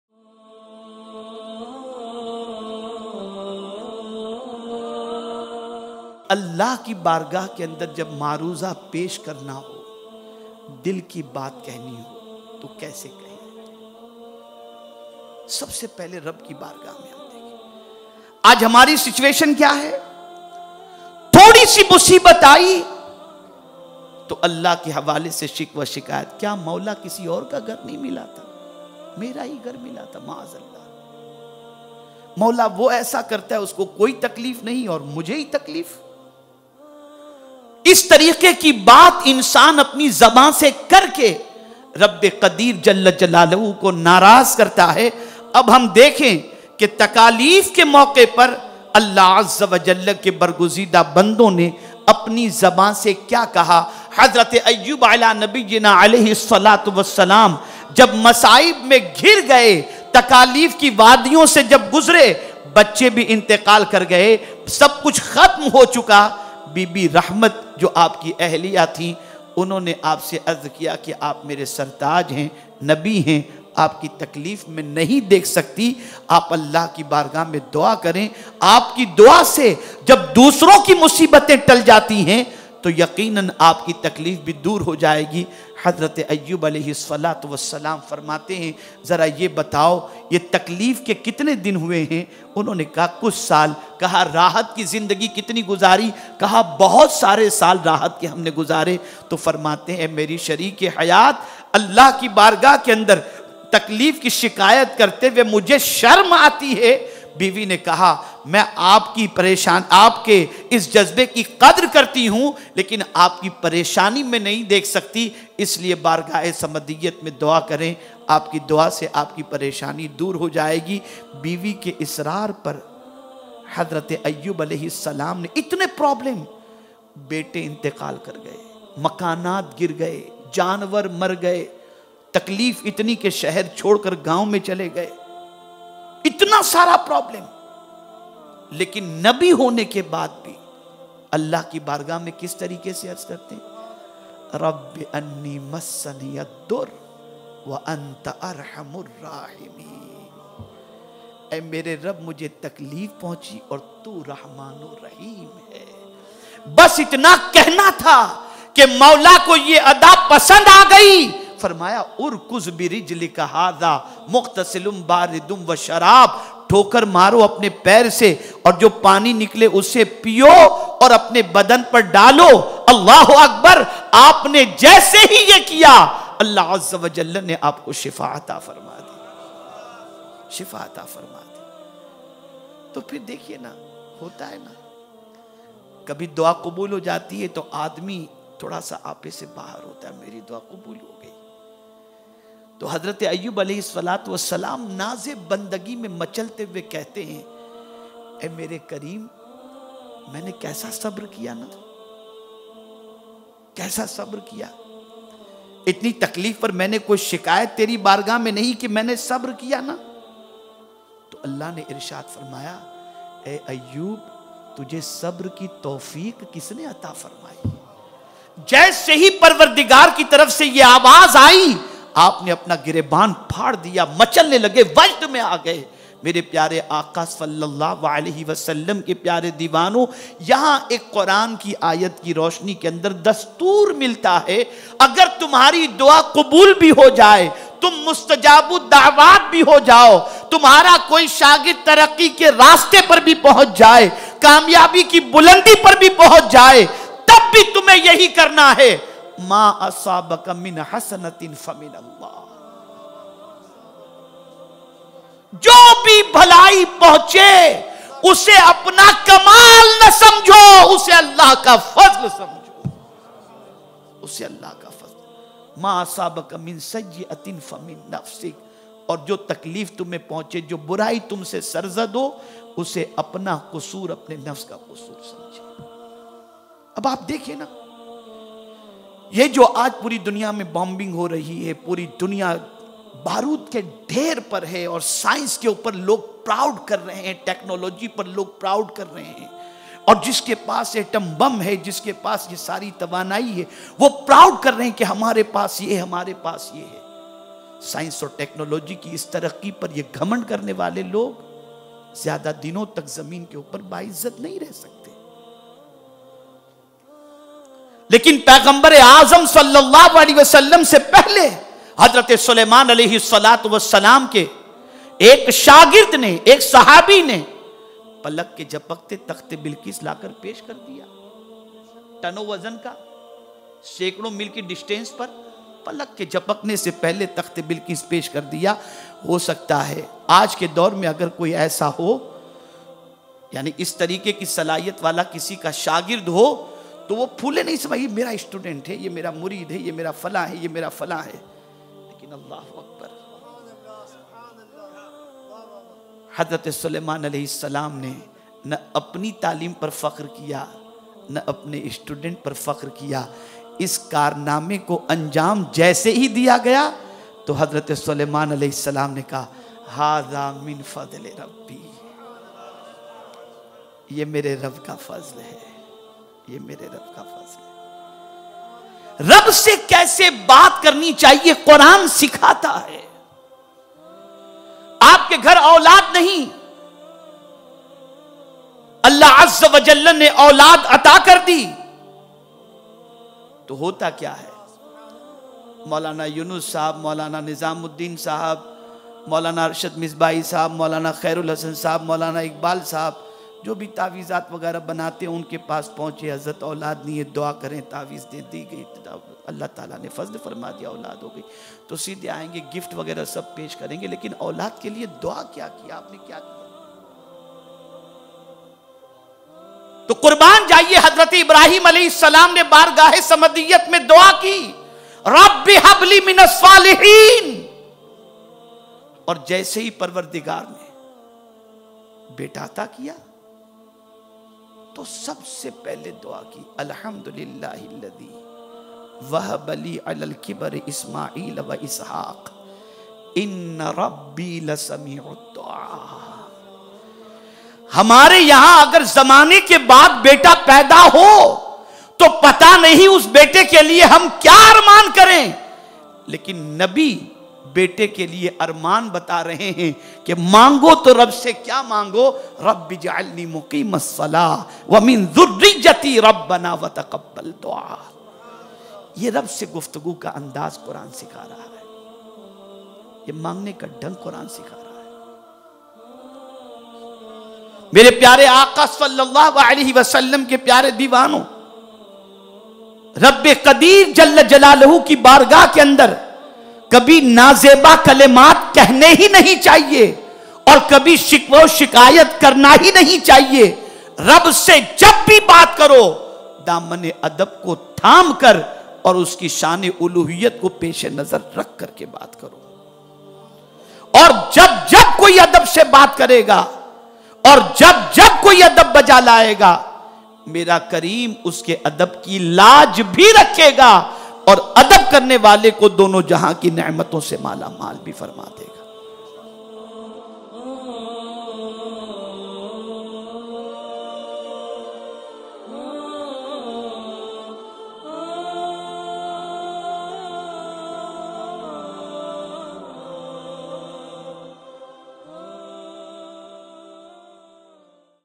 HOW TO BESEECH YOUR LORD - (English subtitles)
HOW TO BESEECH YOUR LORD
(English Subtitles)

Such an amazing speech by Hazrat Ameer E Sunni Dawate Islami Hafiz o Qari Allama Moulana Shakir Noorie, on how to supplicate in the court of Allah ﷻ and how important it is to be grateful for those precious dua's which have been accepted.

Hazrat refers to the beautiful example of
Prophet Ayub عليه السلام
and his faithful wife
Rahima رضي الله عنها,
who are resting on the outskirts of Sanli Urfa (Turkey).